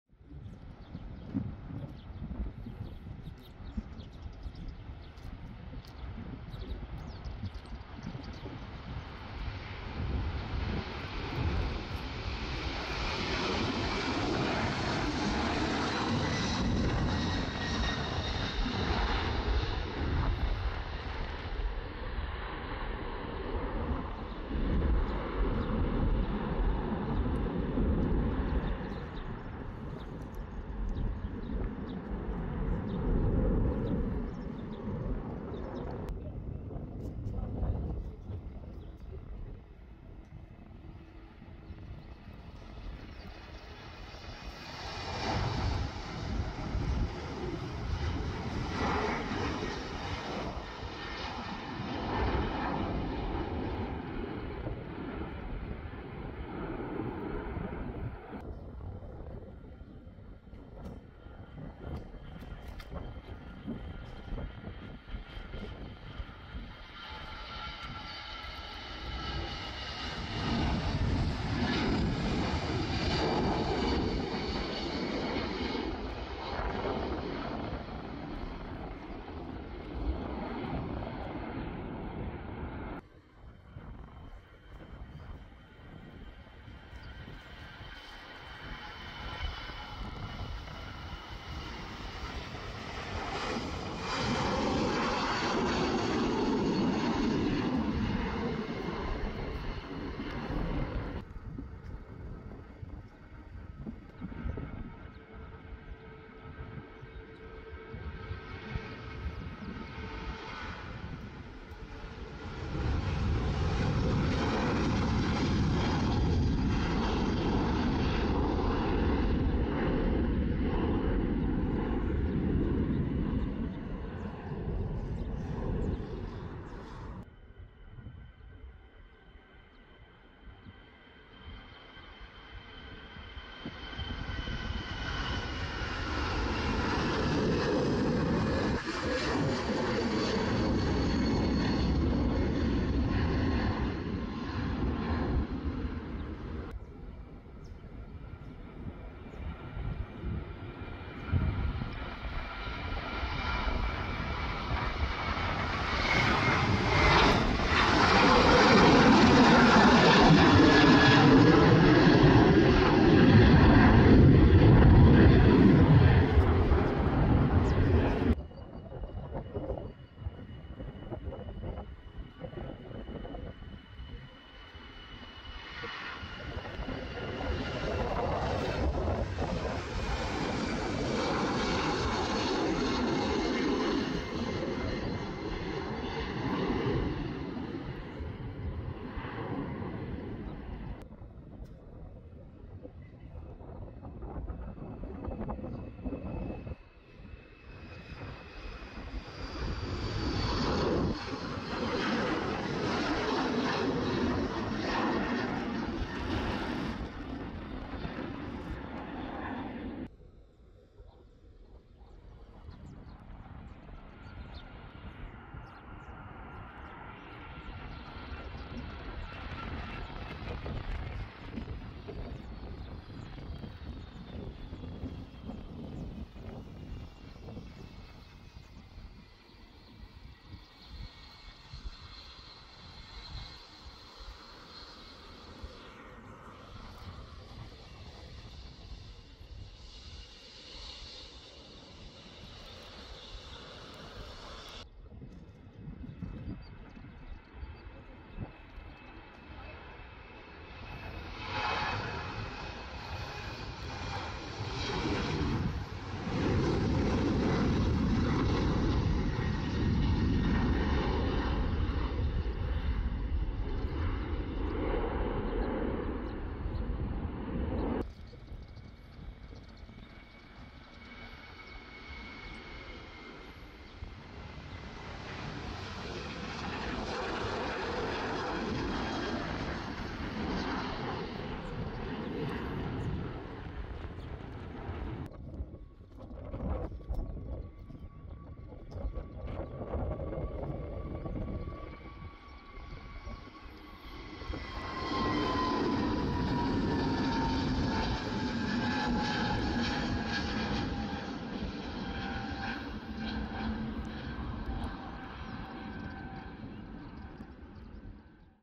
Plane Spotting at Lisbon Airport - Incredible Takeoff in 5 Minutes
Today I Am showing Plane Spotting At Lisbon International Airport I Hope You Enjoyed Too